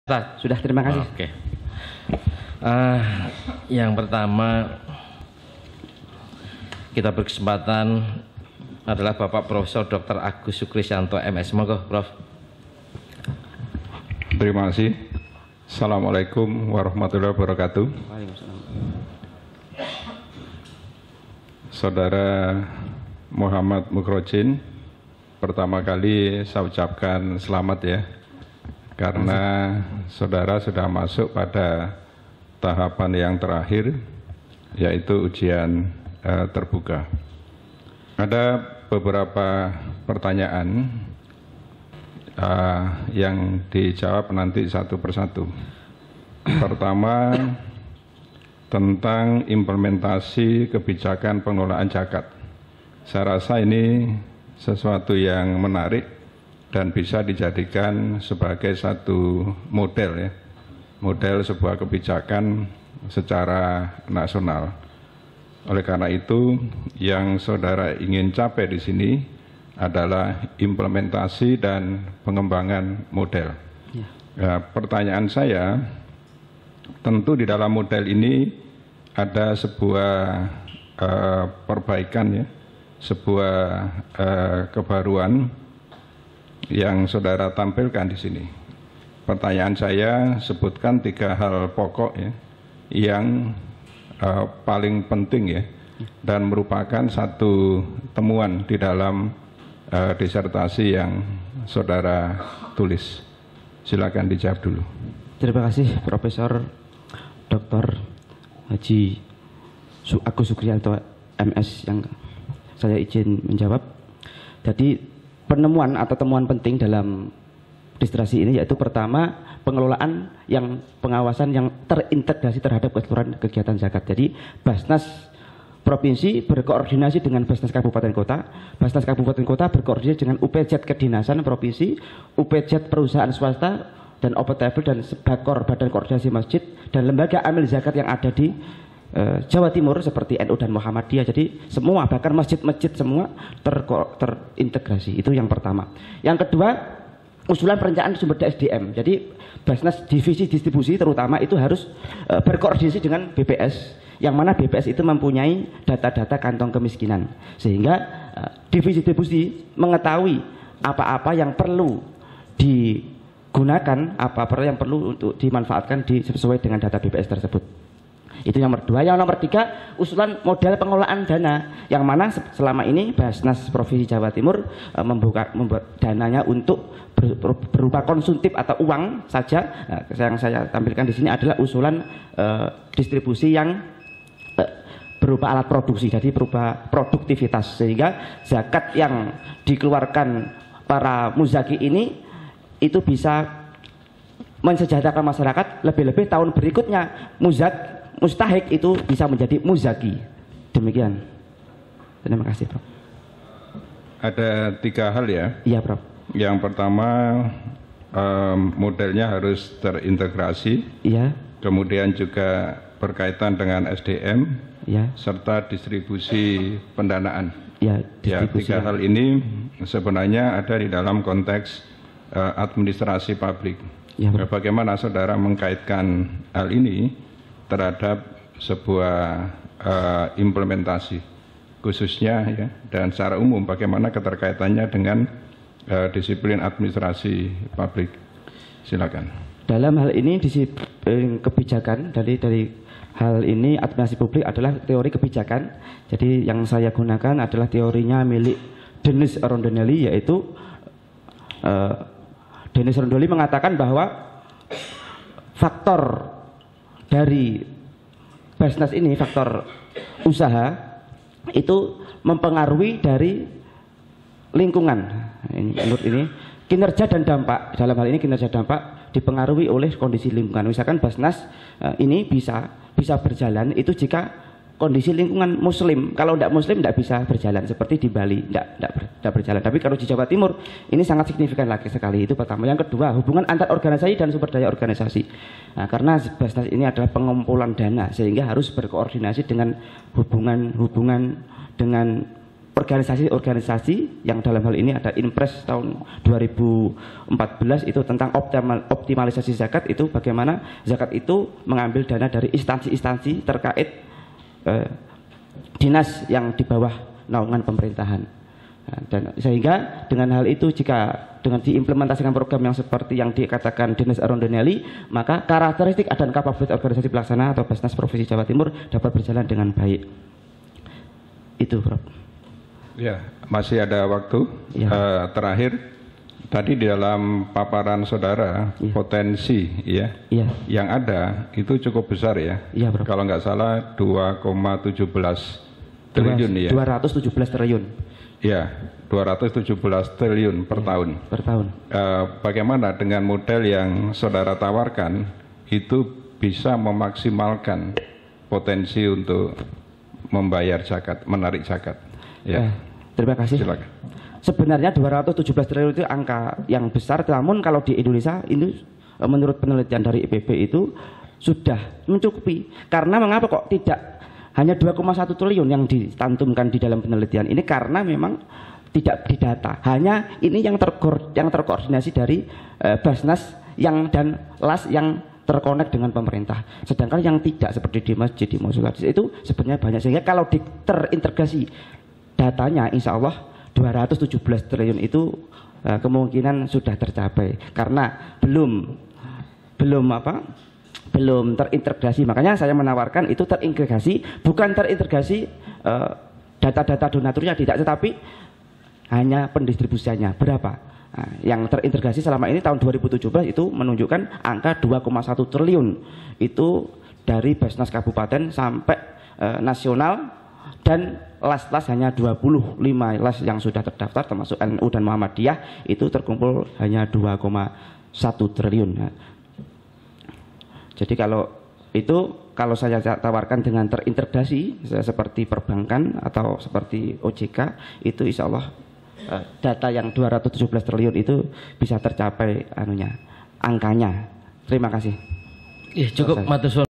Prof. Dr. Agus Sukristyanto, MS menguji Disertasi Dr. Moh. Mukhrojin, S.Pd.I, SH, M.Si
berikut adalah ujian terbuka Disertasi Dr. Moh. Mukhrojin, S.Pd. I,SH, M.Si